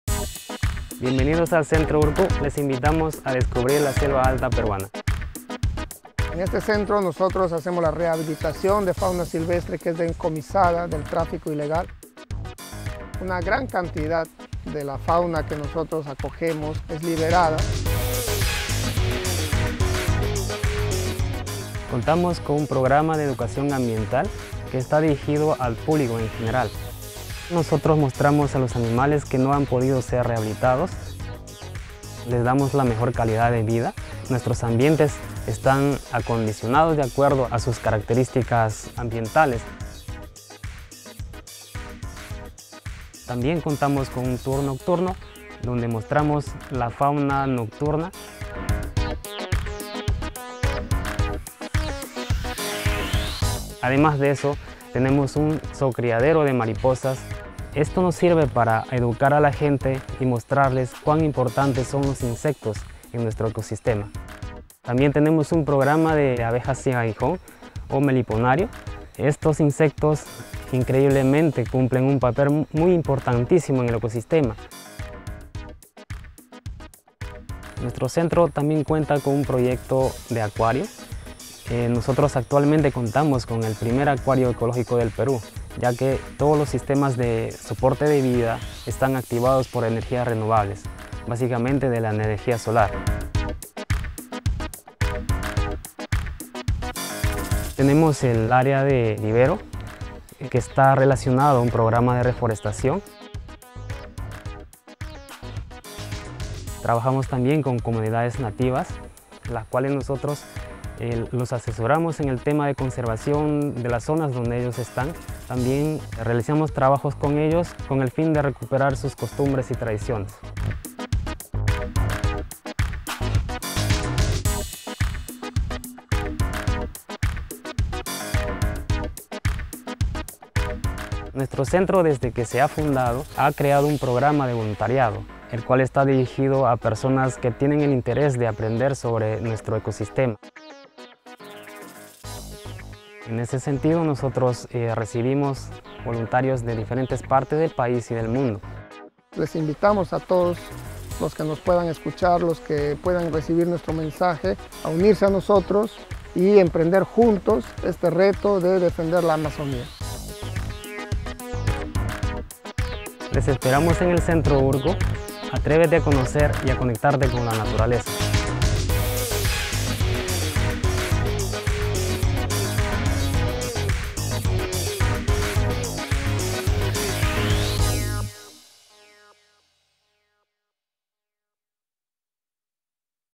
URKU Center in Tarapoto - the place to learn about the amazing Peruvian High Jungle.
Wildlife rescue and rehabilitation, butterfly breeding, ecological community farming - the amazing people of URKU do all those things and many more.